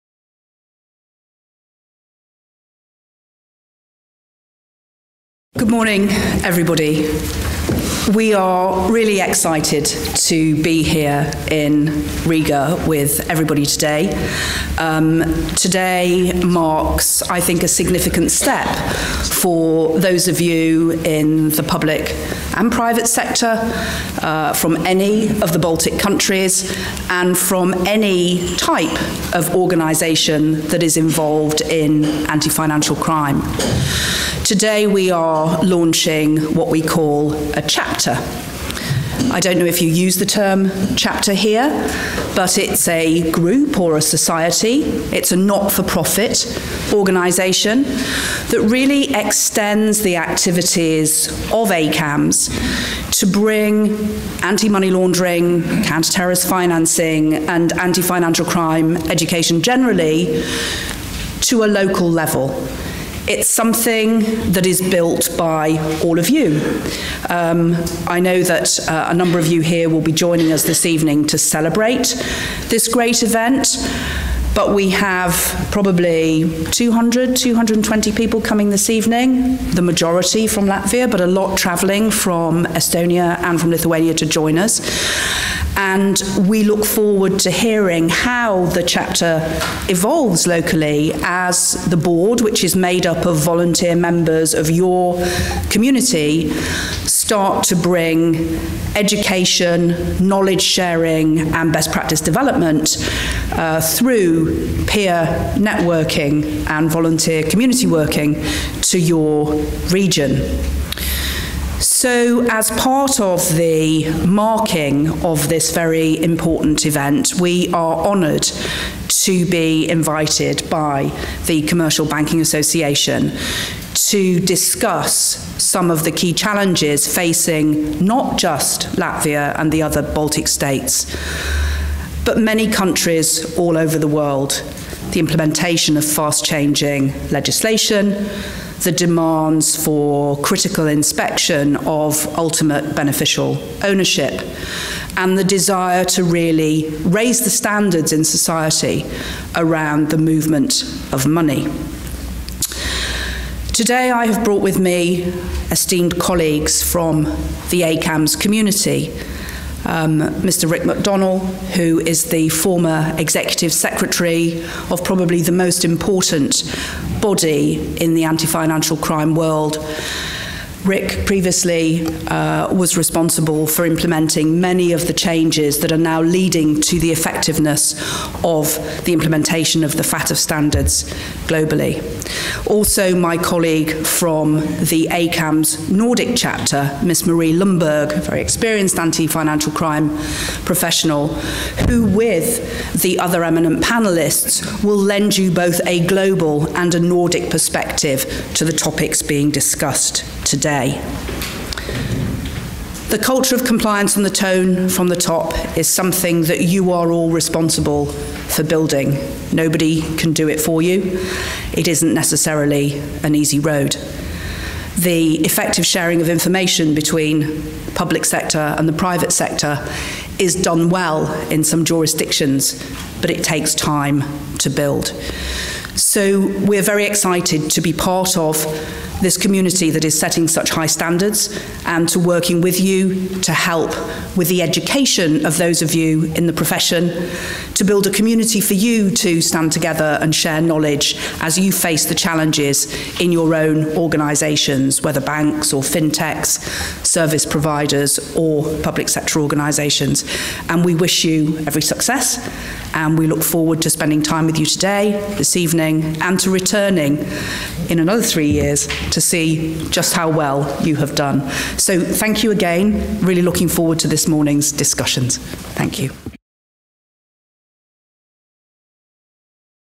Opening speech by Angela Salter, Head of Europe, ACAMS, 08/11/2017
High-Level Workshop on AML/CFT Compliance, November 8, 2017

"Building Culture of Compliance and Effective Public-Private Partnerships to Combal Financial Crime" | Riga, Latvia